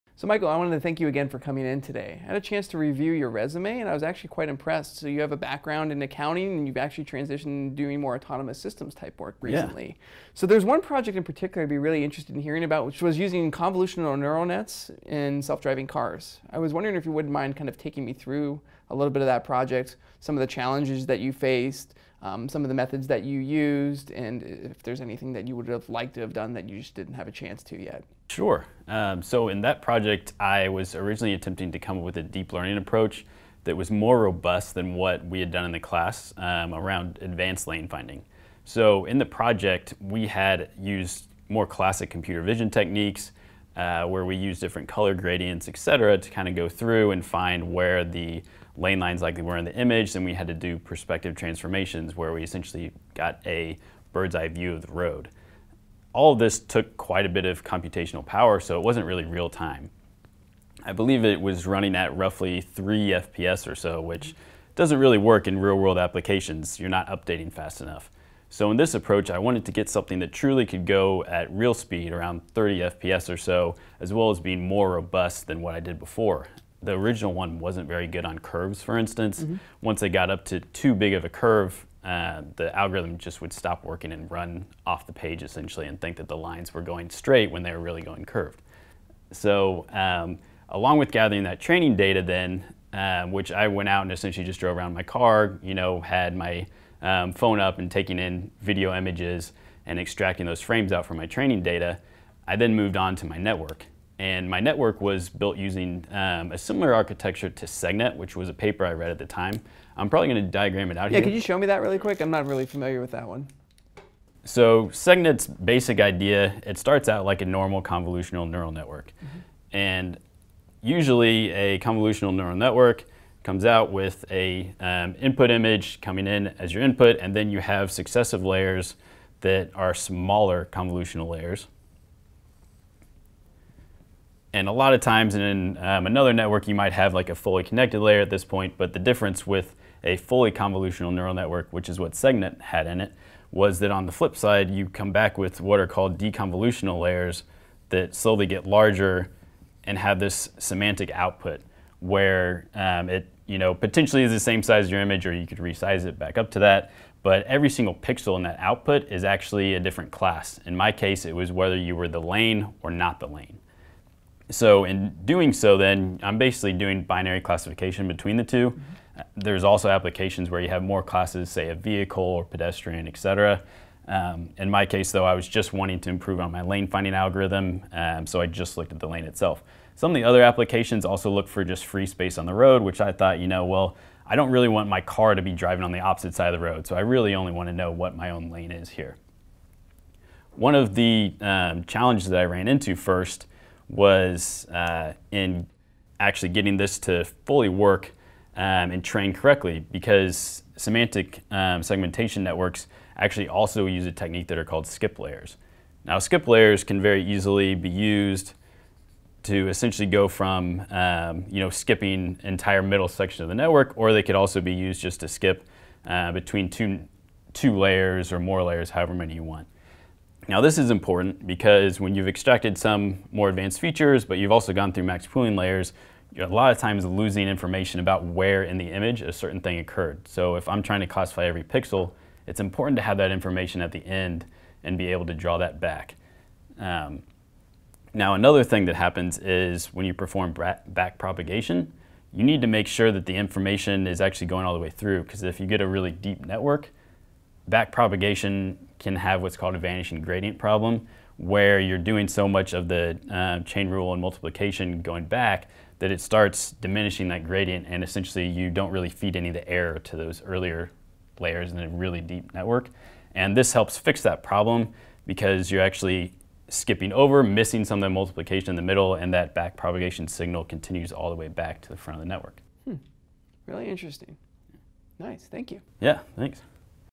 My Project
In this walkthrough, learn how to walk a hiring manager through the artificial intelligence and deep learning projects that make up your portfolio.

Start learning the skills needed for these roles in Udacity's School of Autonomous Systems